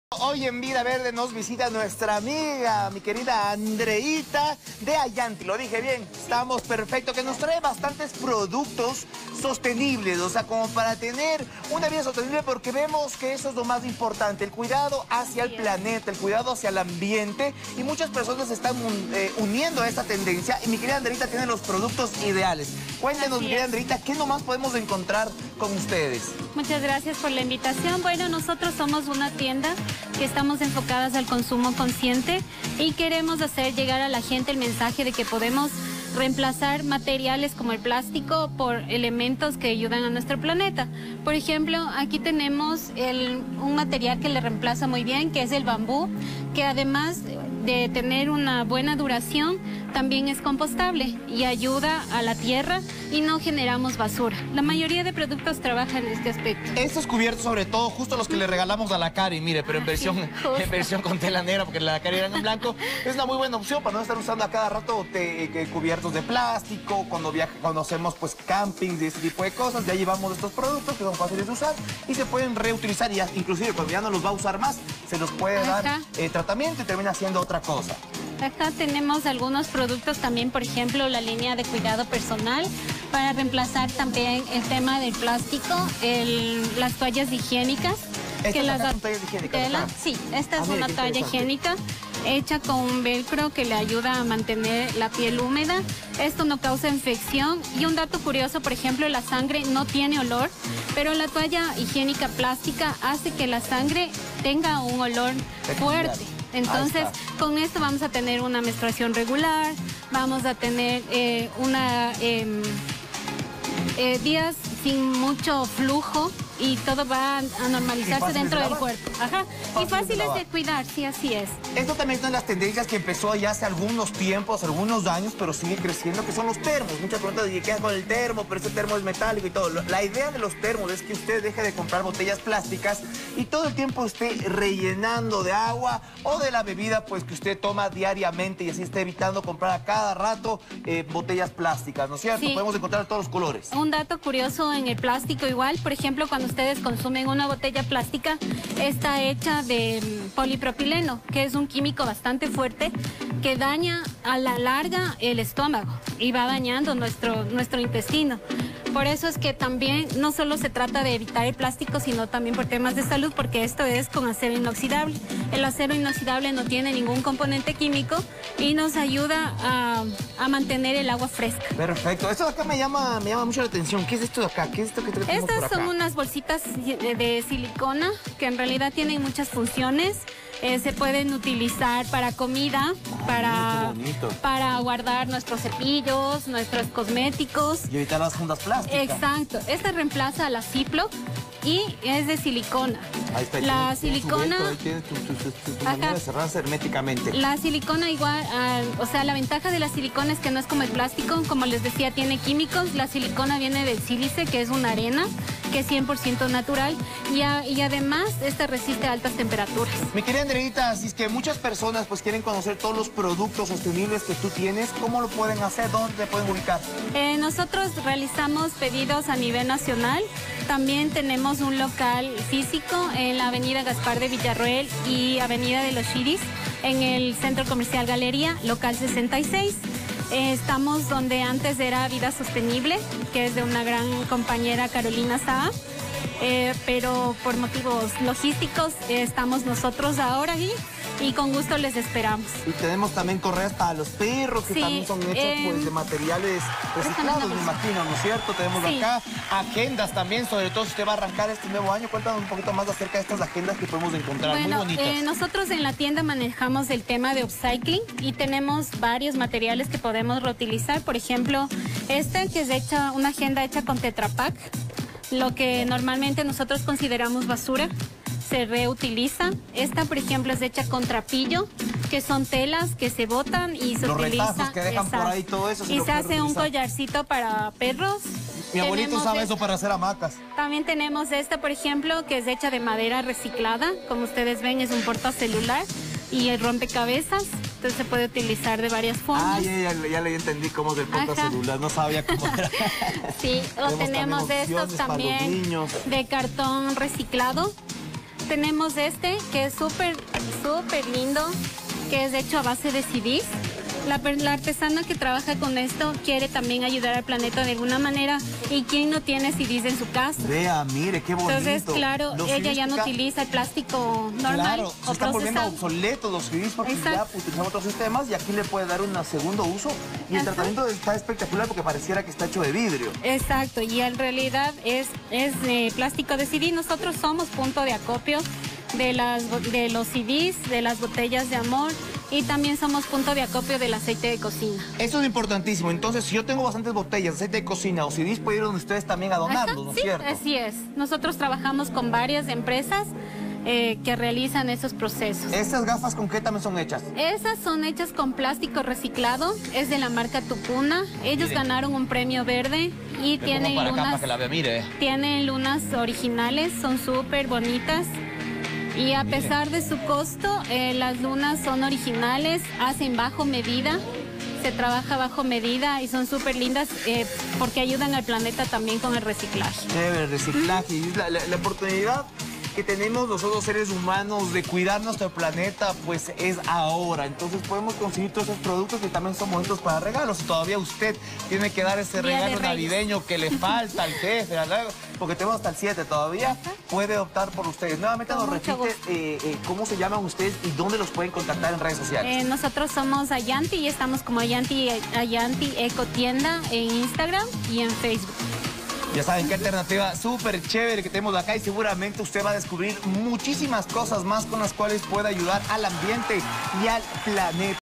Revisamos productos amigables con el medio ambiente
La mañana de este martes, Andrea Altamirano nos mostró los productos para que hogar que ayudan a cuidar el medio ambiente y evitar daños a la naturaleza.